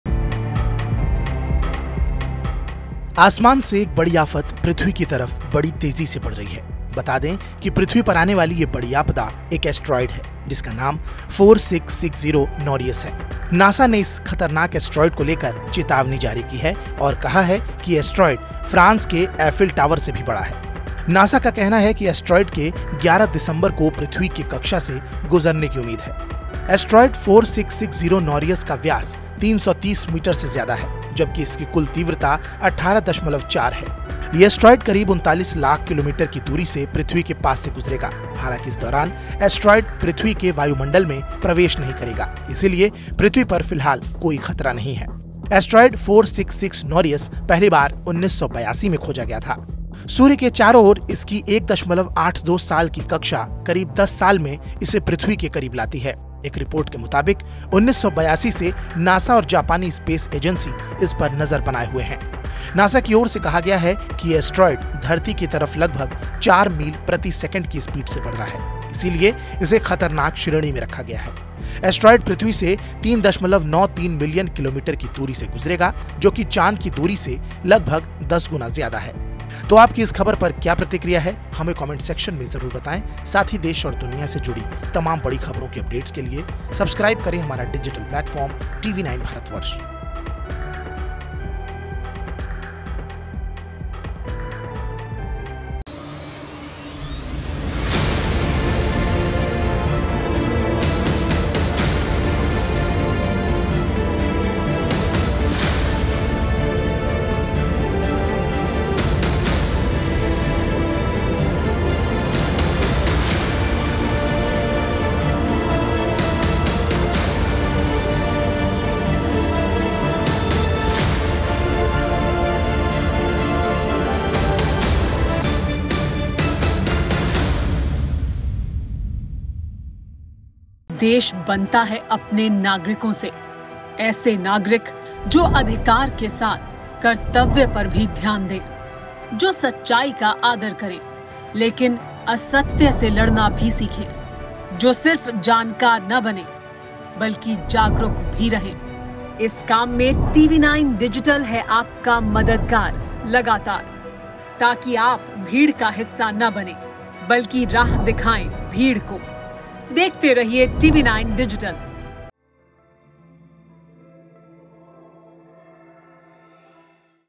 NASA ने जारी की चेतावनी, आसमान से धरती की ओर आ रही है बड़ी तबाही! | Asteroids 4660 Nereus
आसमान से एक बड़ी आफत पृथ्वी की तरफ तेजी से बढ़ रही है. बता दें कि पृथ्वी पर आने वाली ये बड़ी आपदा एक एस्टेरॉयड है. जिसका नाम 4660 नारियस है, नासा ने इस खतरनाक एस्टेरॉयड को लेकर चेतावनी जारी है और कहा है कि ये एस्टेरॉयड फ्रांस के एफिल टावर से भी बड़ा है. नासा का कहना है कि एस्टेरॉयड के 11 दिसंबर को पृथ्वी की कक्षा से गुजरने की उम्मीद है.

Credit: International | Pushpanjali/Producer | Harendra/Editor